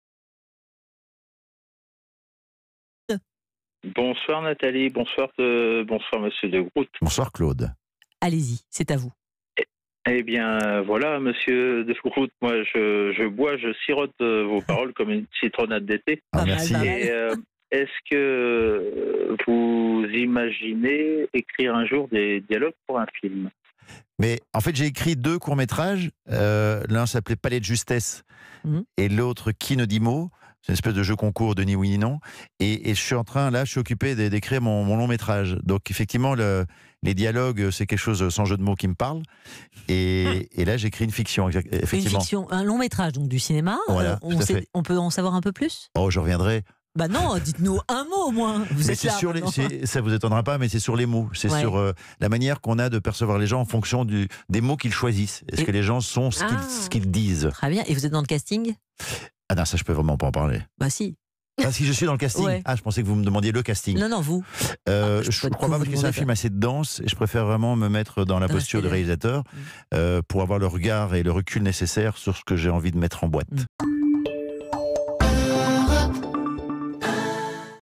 Stéphane de Groodt : "Mon premier long métrage est sur les mots"
L'auteur et acteur Stéphane de Groodt s'est lancé dans l'écriture d'un premier long métrage. Il explique jeudi sur Europe 1 que celui-ci portera "sur les mots, la manière qu'on a de percevoir les gens en fonction des mots qu'ils choisissent".